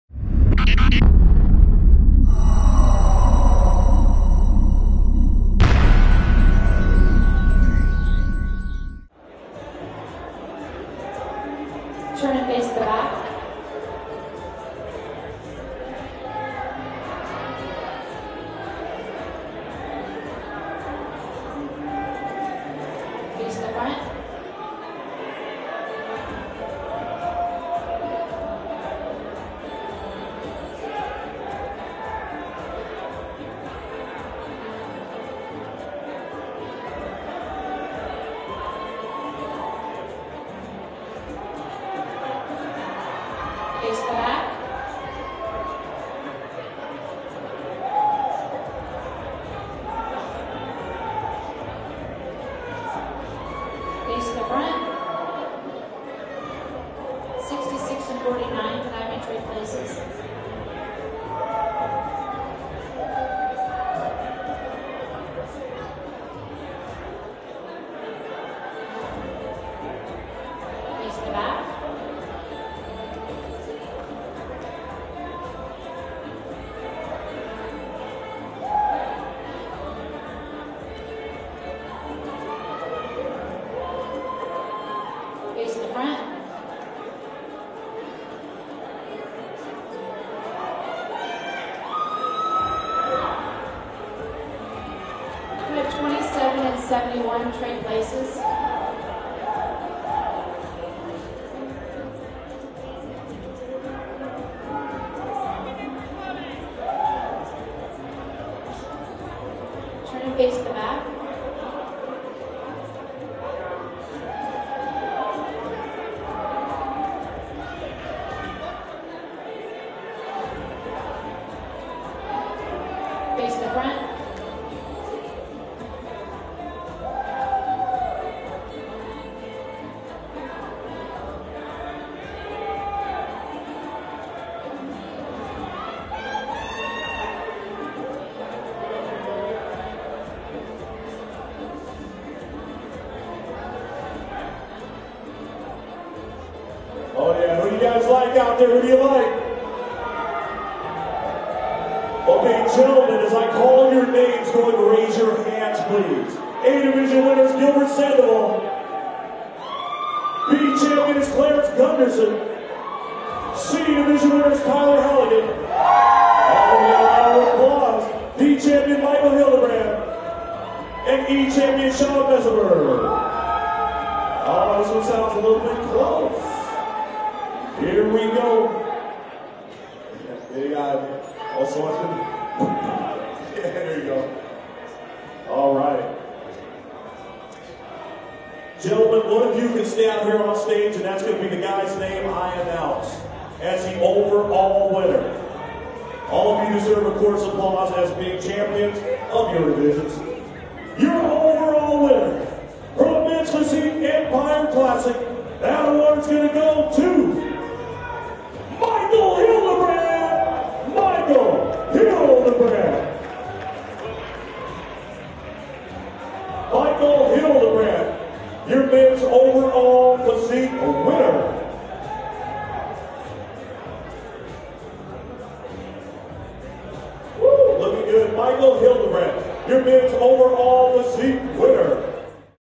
2015 Empire Classic Open Men Physique Overall
2015 Empire Classic Open Men Physique Overall. May 1st at the Northern Quest Casino in Spokane WA. A Ribic Production.